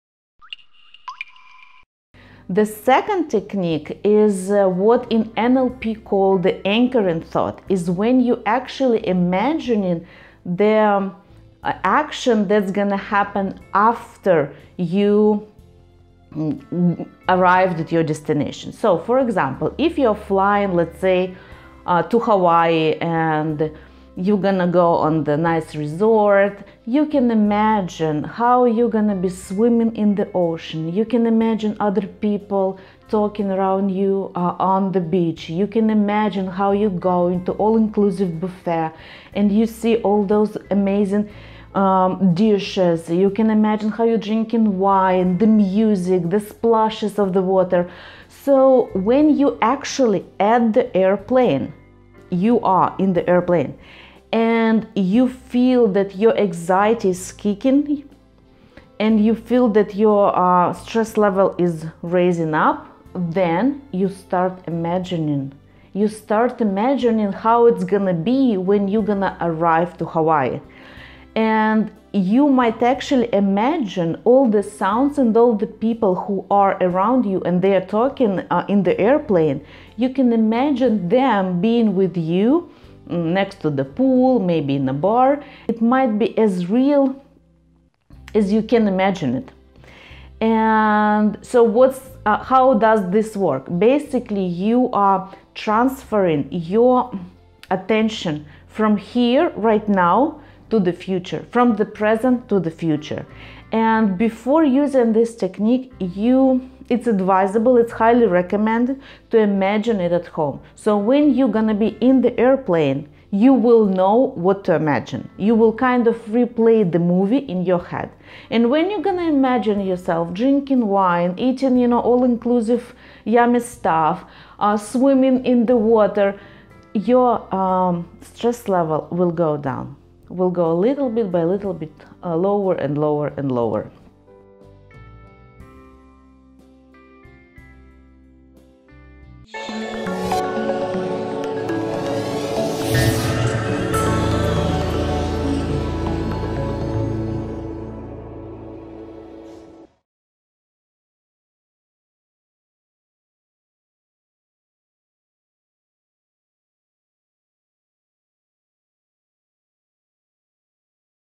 How to Overcome Fear with an Anchoring Effect. NLP Psychology by the Life Coach Elena Semenek
How to overcome fear with an Anchoring Effect. NLP Psychology by the life coach Elena Semenek. Motivational video on how to overcome fears.

Elena Semenek,
The founder of Psychology of Happiness where happiness is the purpose of life!

🚀 Have a question?